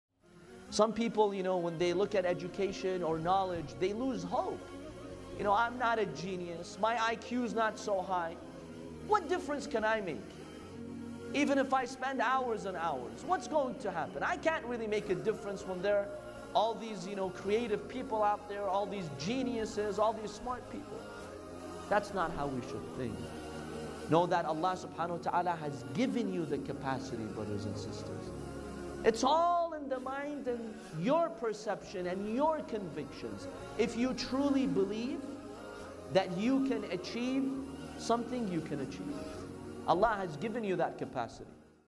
Allah Swt Has Given Us the Capacity to Achieve Anything!
Clip by Sayed Mohammad Baqer Qazwini